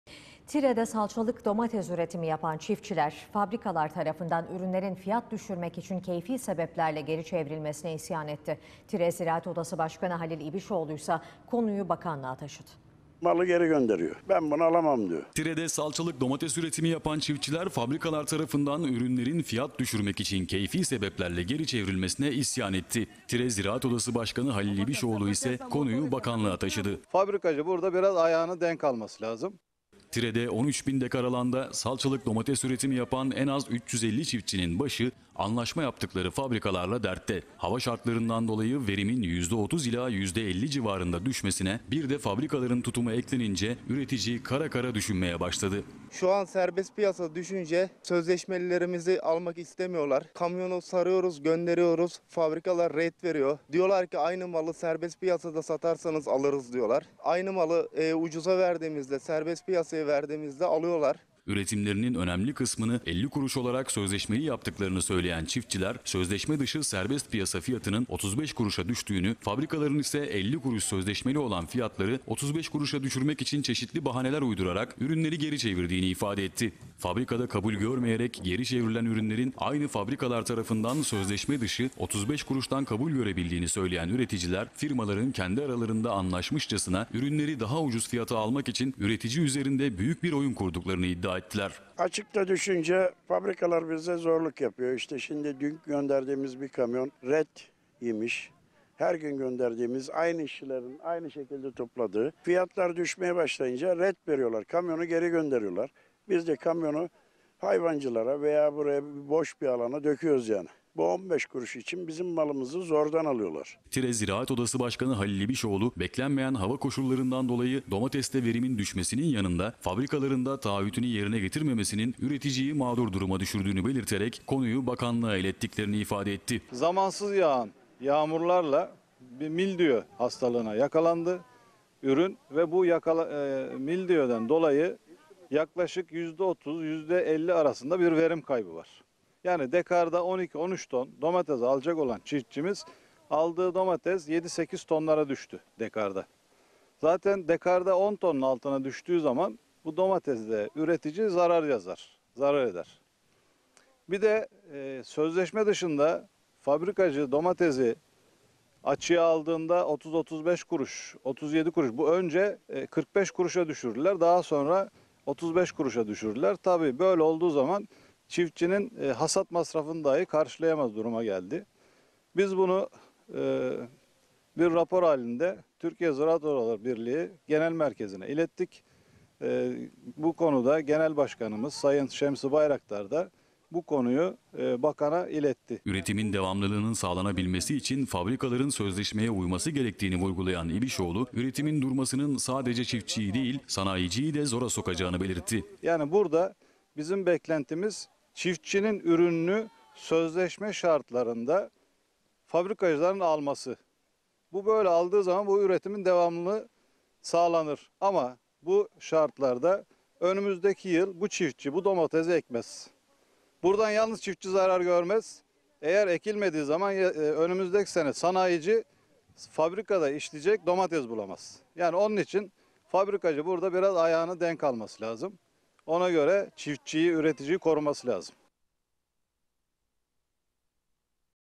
Tireli domates üreticisinin isyanı TVDEN'de
HaberTire'nin gündeme taşıdığı, Tireli domates üreticisinin isyanı TVDEN ekranlarında böyle yer aldı. Ercan ÇELİK'in özel haberi.